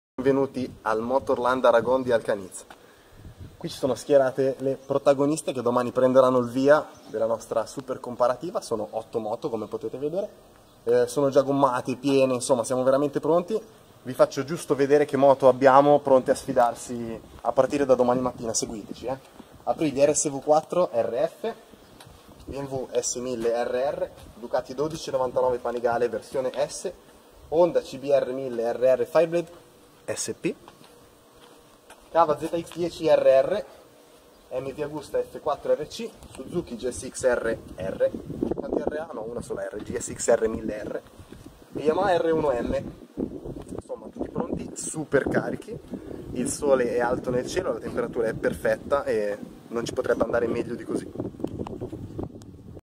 Comparativa maxi sportive 2017: le protagoniste della sfida ad Alcaniz
Aprilia RSV4 RF, BMW S 1000 RR, Ducati 1299 Panigale S, Honda CBR1000RR SP, Kawasaki ZX-10RR, MV Agusta F4 RC, Suzuki GSX-R1000R e Yamaha R1M: sono queste le protagoniste della comparativa maxi sportive 2017 di Motociclismo. Le moto si sfidano in pista al Motorland Aragon di Alcaniz